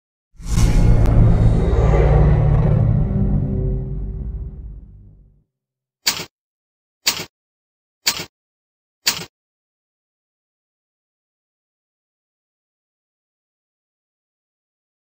Sain
Трейлер канала.

И да, фанаты SFM анимации, звук в начале взят из вступления Source Filmmaker и звук принадлежит им и только им.